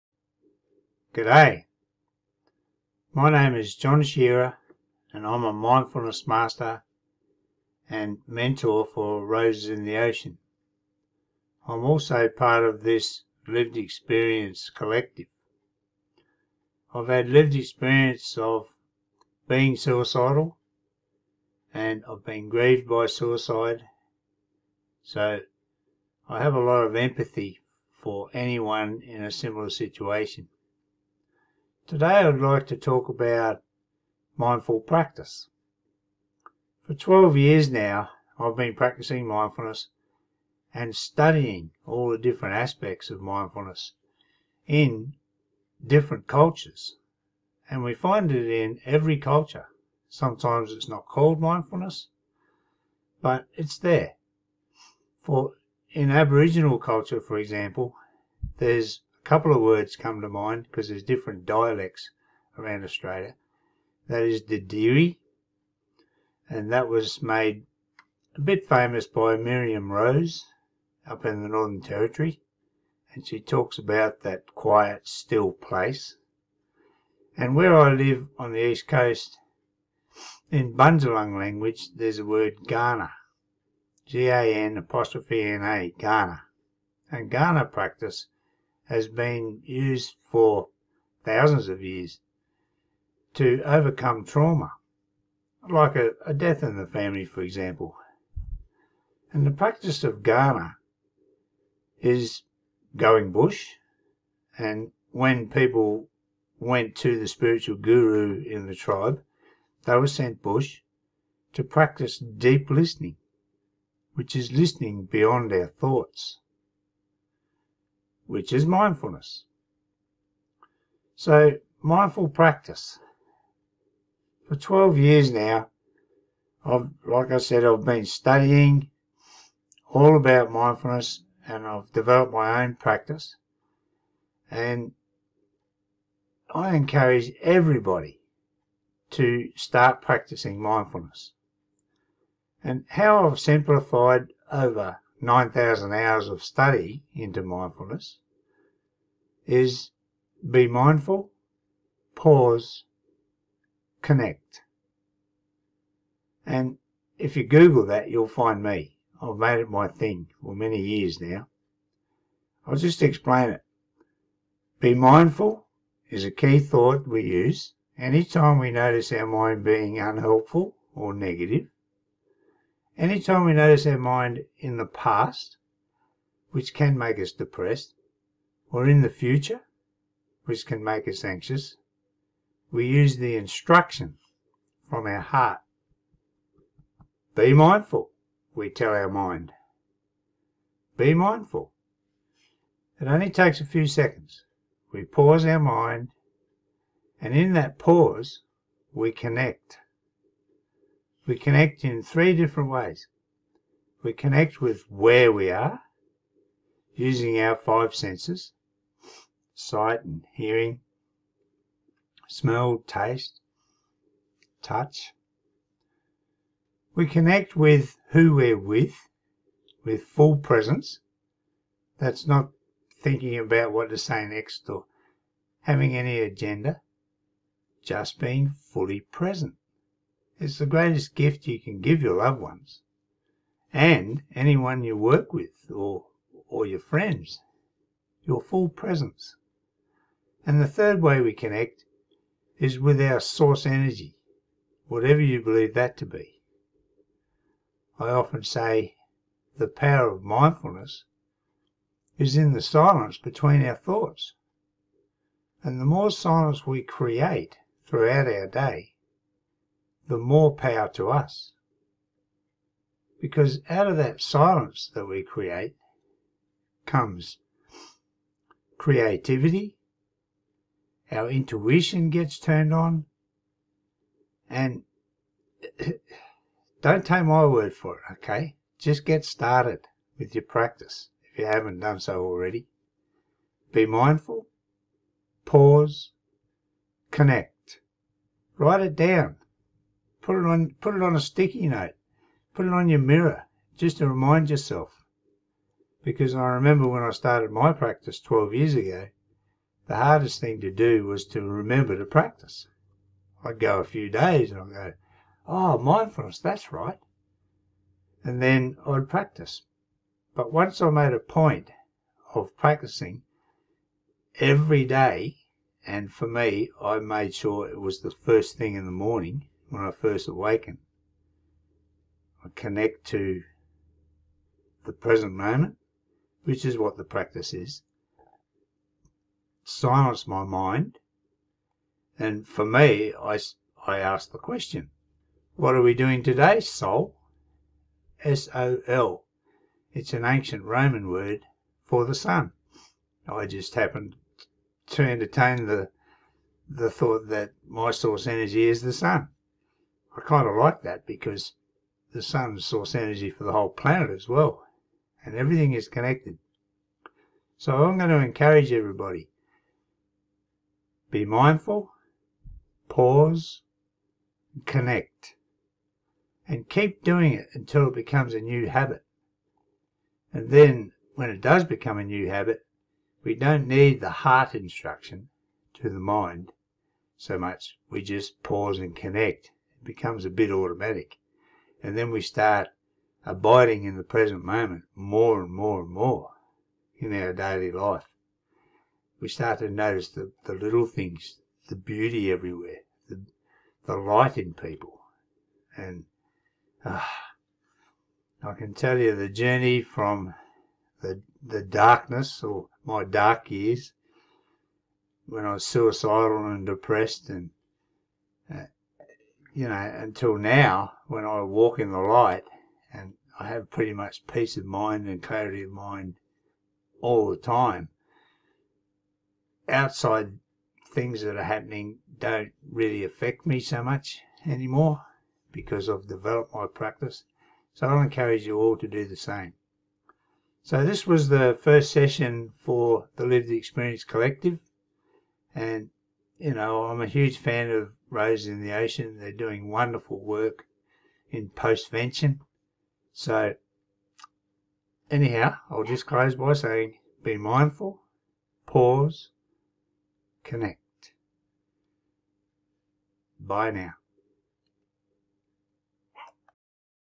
Mindfulness with John Shearer - Session 1
Be mindful, pause and connect every fortnight with mindfulness master, John Shearer.